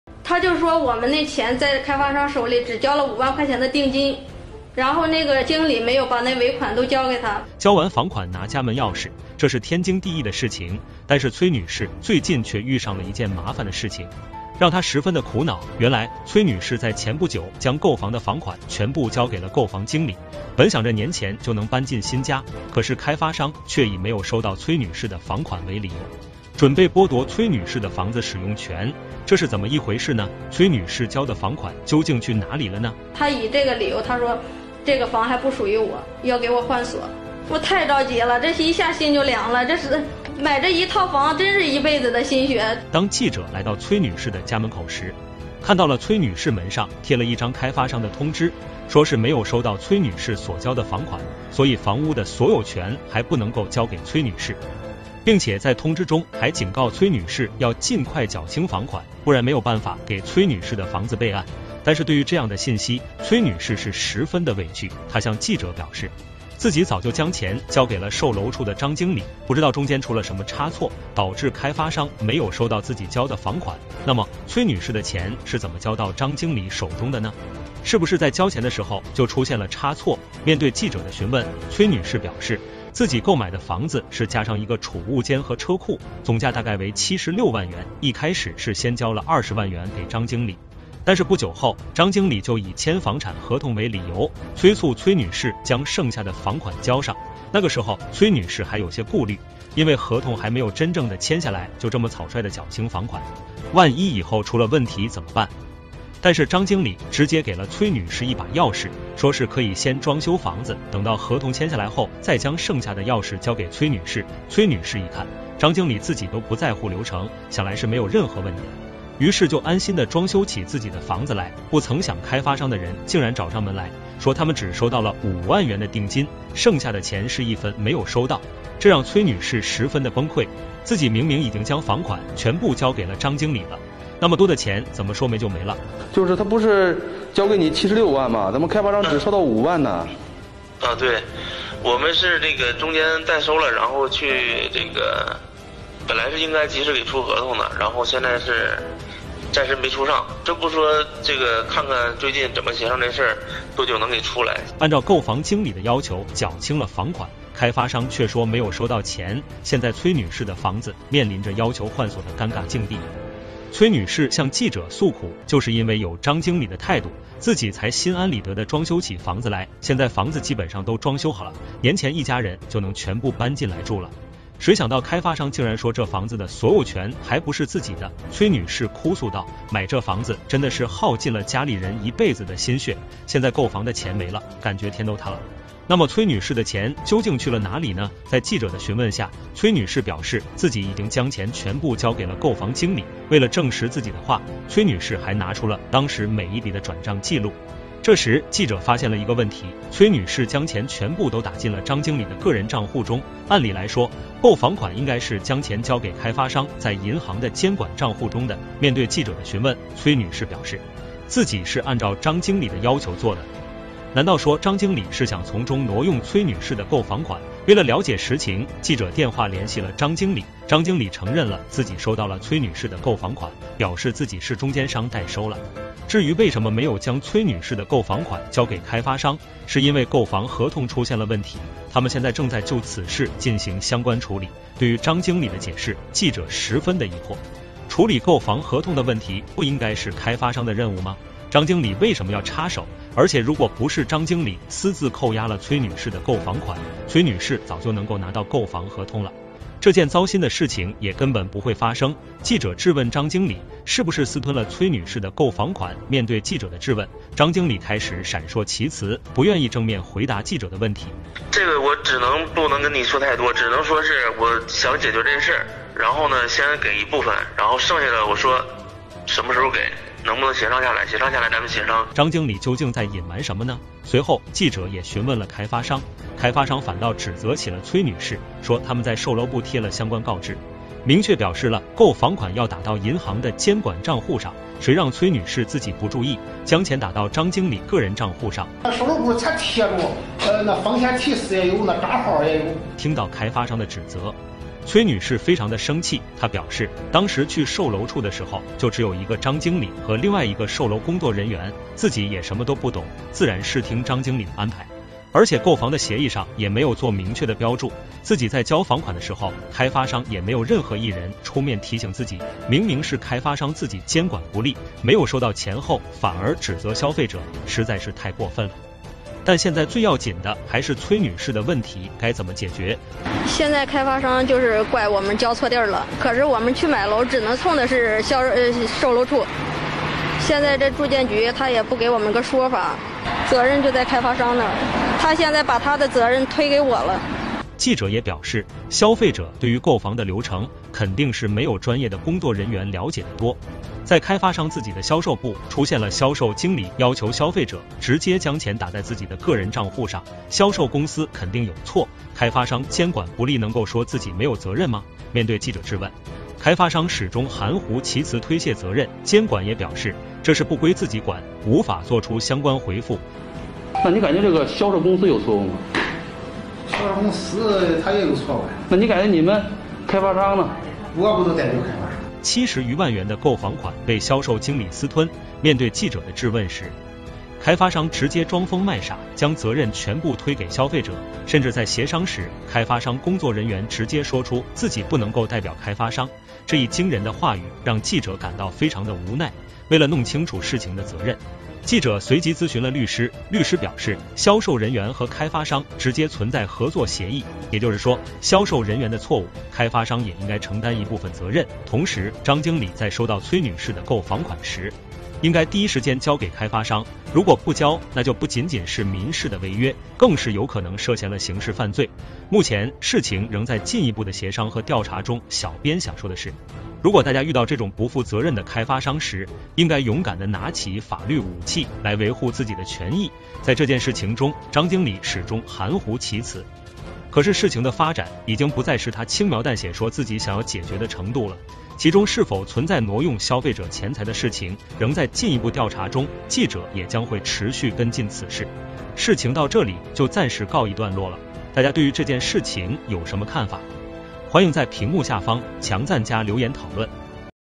76万购房款交给售房经理，开发商却换锁不给房，女子：一辈子心血
NEWS 新闻 - 76万购房款交给售房经理，开发商却换锁不给房，女子：一辈子心血